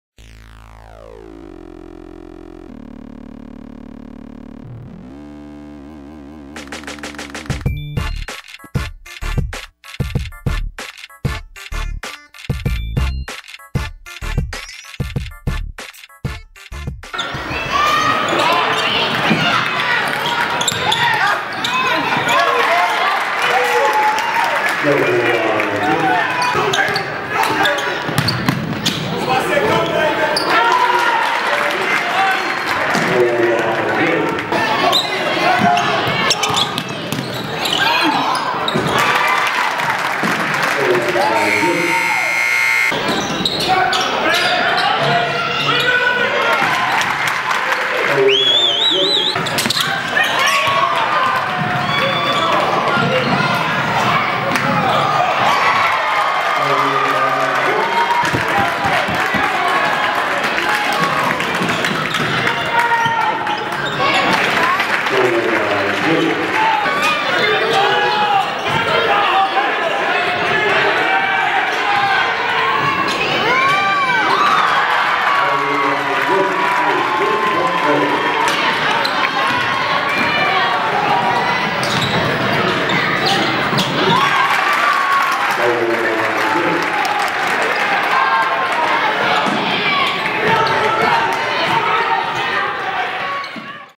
Quentin Goodin Gets 26 Points At 2015 KOBGS
- Taylor County High School Cardinals junior guard Quentin Goodin scores 26 points and grabs 8 rebounds in the 83-80 victory over #2 ranked Henderson County High School in the 2015 King of the Bluegrass Showcase.